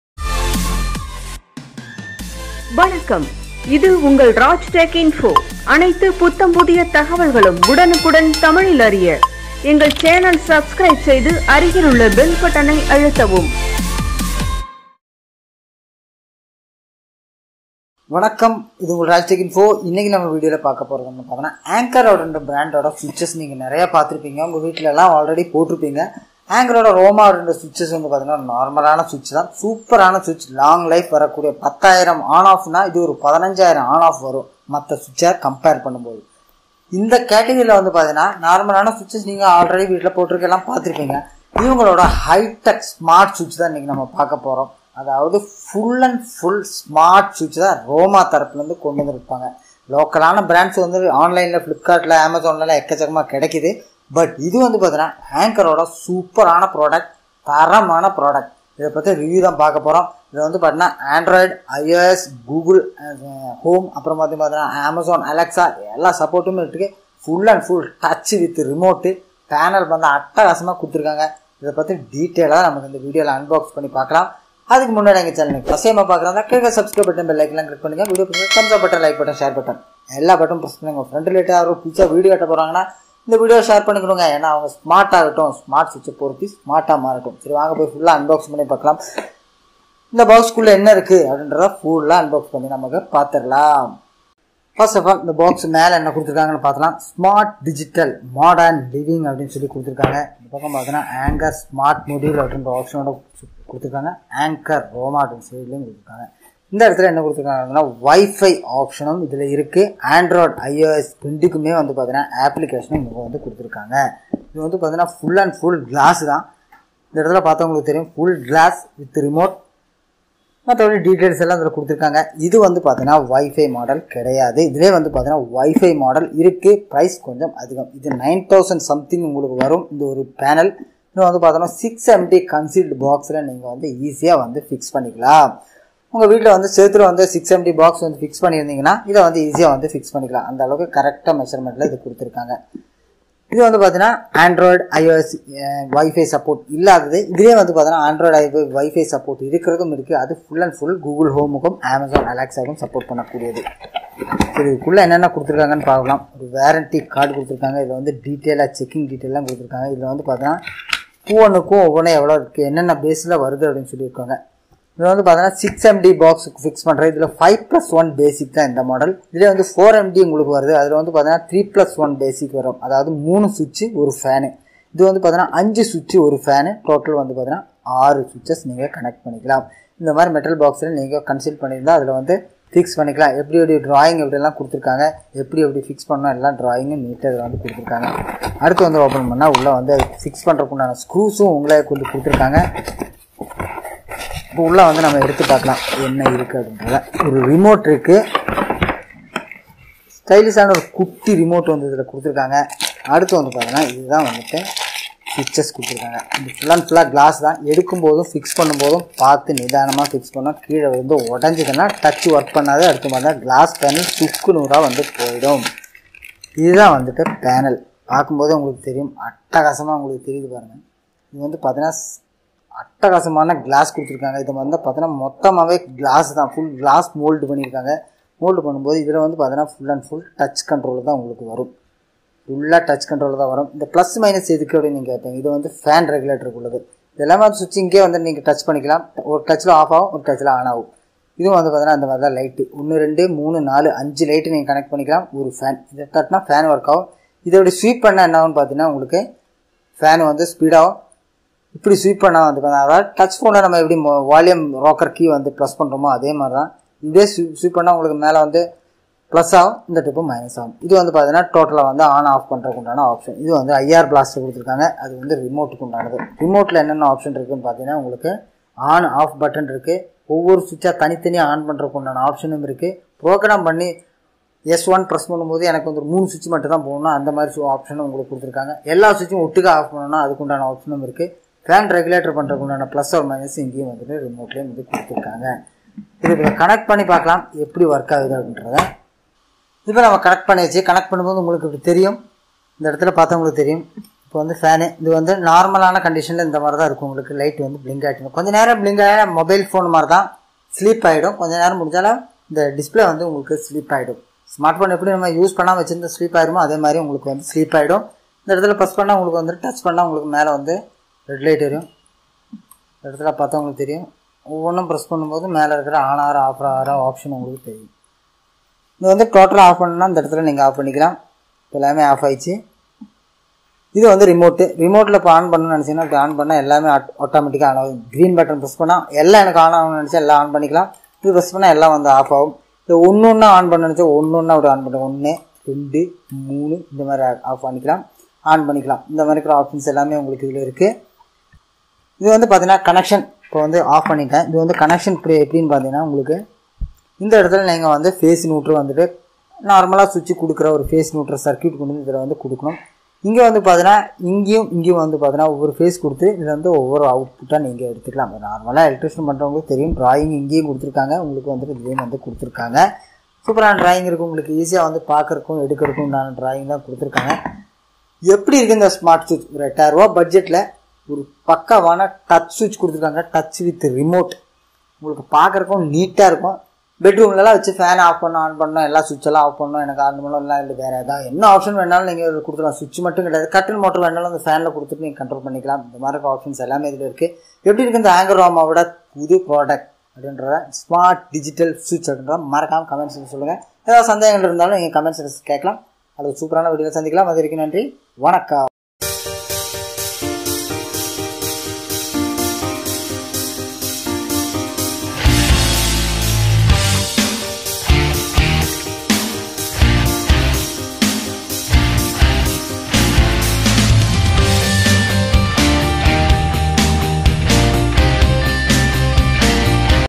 Anchor Roma Smart Home Switch/Full Touch Controls/Remote Access/ Wifi and Android iOS/Demo Video
Hi friends this video we speak about Anchor Roma Smart Home Switch... Full Specifications and Working Deatails and Demo...

Watch Video ,please share.

Our  Share Chat App link for Virel content..